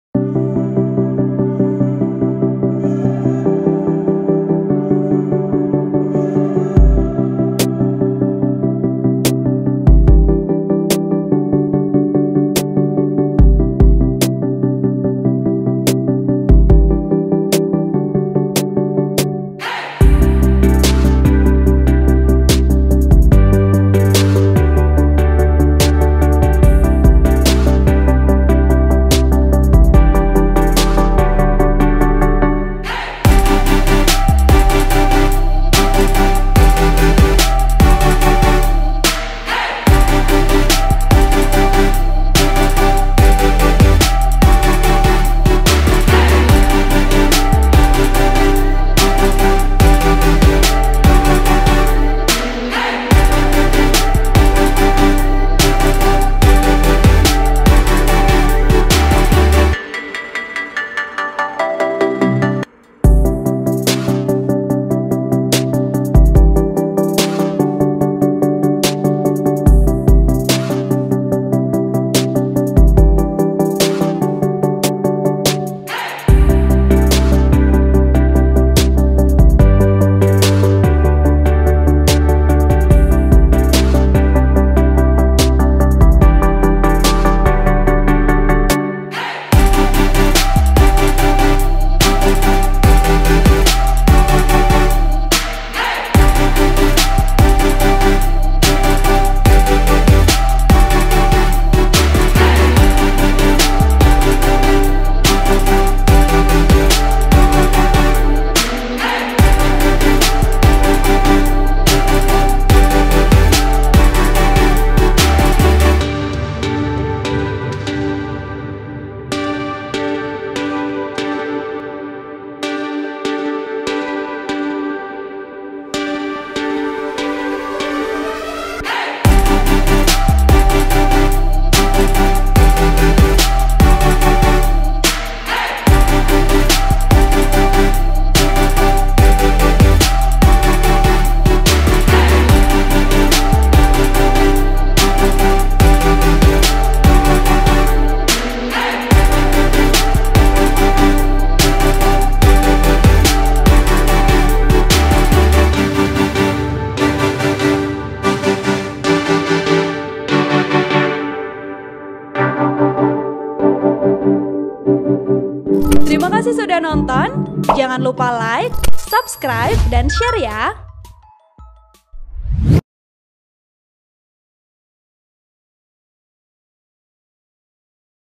Jordi Amat Absen, Egy yang Sempat Cedera Jelang Laga Indonesia vs Filipina
Jordi Amat Absen Hingga Egy Yang Sempat Cedera Jelang Laga Indonesia vs Filipina di Piala AFF 2022

(OPA)